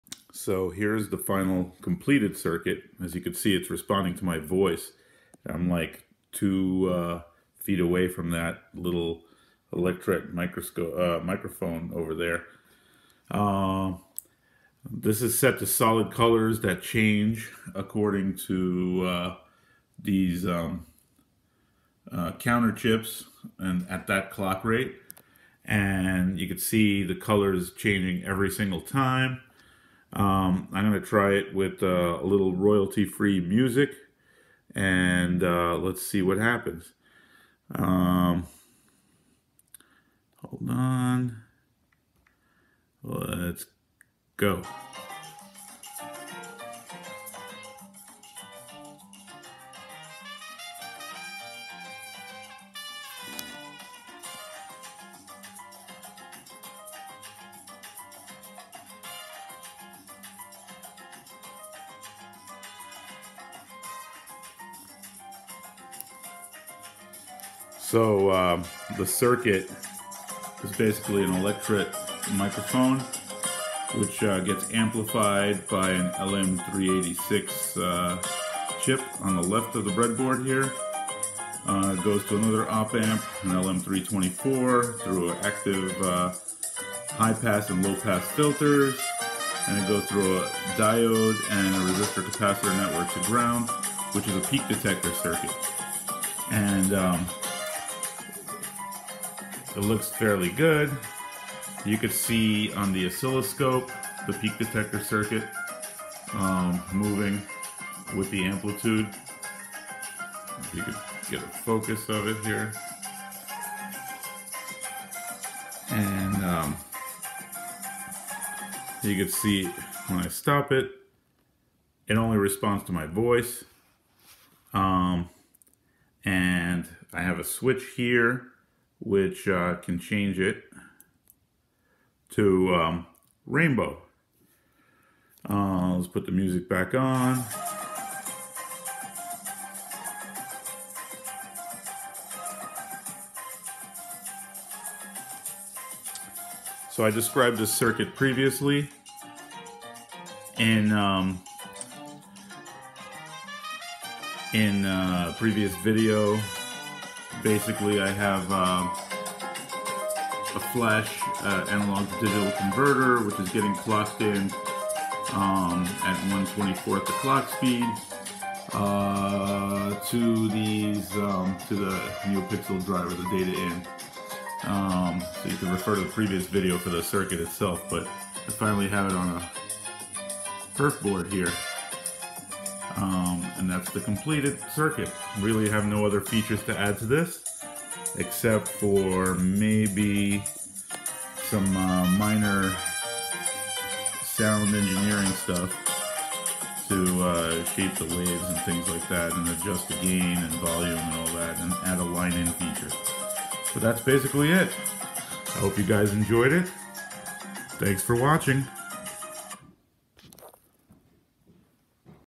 Audio responsive neopixels completed!
Demo of audio input op amp amplifier and peak detector circuit going to data input of the all-hardware neopixel driver. Prior to this the auto cycling color input module is ANDed with the audio input signal to give a prettier neopixel display. With this and with the exception of some minor audio input feature adjustments (audio line-in and maybe bluetooth) the neopixel without coding project is complete!

The project uses all timers counters and shift registers. There is no microcontroller or coding needed.

neopixel without coding